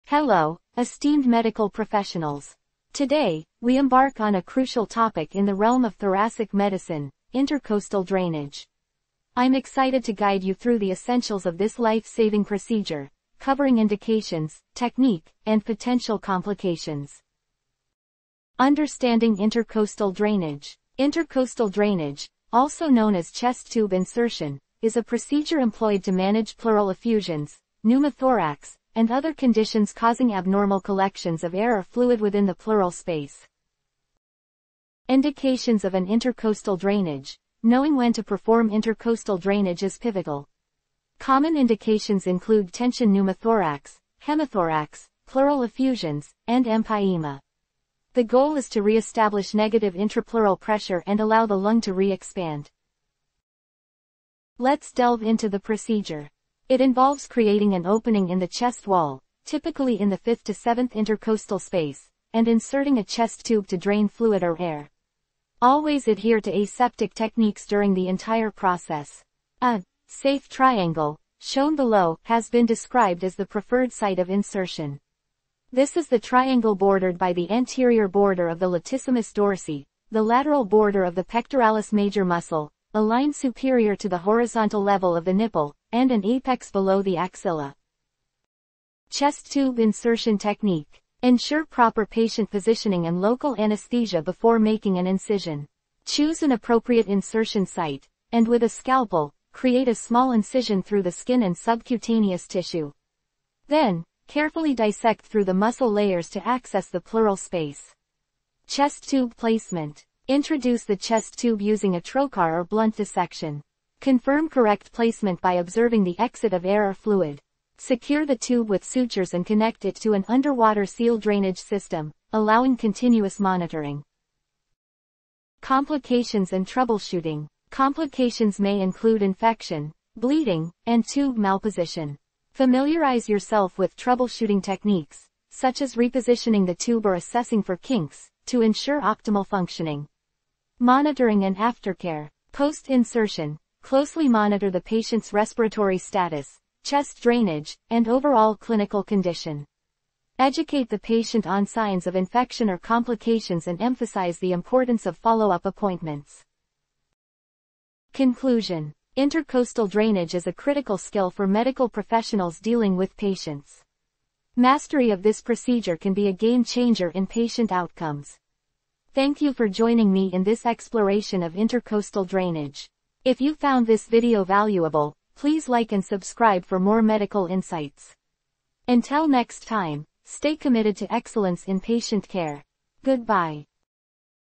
Mastering Intercostal Drainage: Techniques, Indications, and Essential Tips
"Gain expertise in Intercostal Drainage – from the right techniques to crucial indications. Uncover the essentials for successful procedures in this comprehensive guide. 🏥 MedicalProcedures IntercostalDrainage"